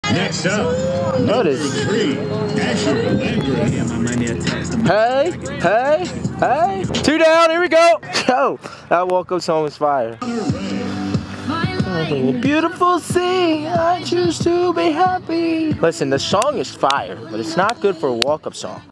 Baseball Coach REACTS To Player Walk-Up Songs MIC’D UP!
Hey Everyone! It’s CS99TV from TikTok! Today Coach Squez reacts to his players walkup songs!